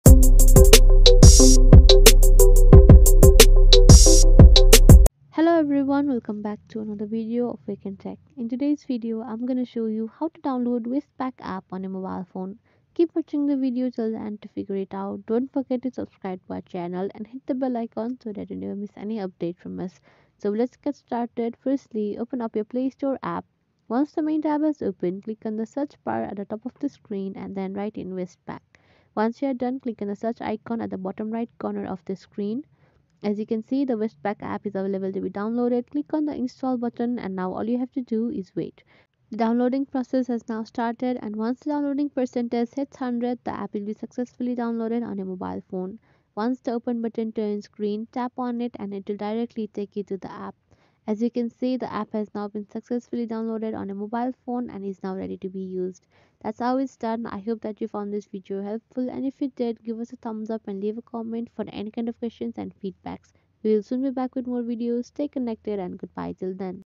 How to Download Westpac App on Android 2023?
Download Westpac App on Android:

Westpac Banking Corporation, known simply as Westpac, is an Australian multinational banking and financial services company headquartered at Westpac Place in Sydney, New South Wales.

With your westpac app installed on your mobile device, you can:
- Make payments
- Make fast and secure payments & transfers or schedule them for the future
- Google Pay™ - Pay with your mobile
- Digital Card - 24/7 access to a digital version of your Westpac card.

Watch the video till the end and follow the steps as shown in it.

0:00 Intro
0:20 Download Westpac App
0:57 Outro